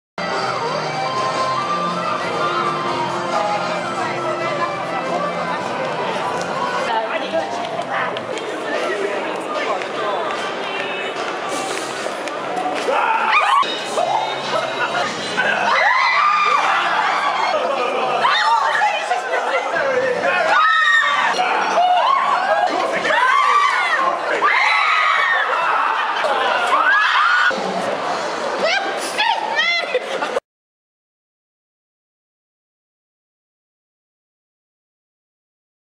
Living Christmas Trees
Previously assumed harmless Norway Spruce become objects of terror as they jump out on unsuspecting guests!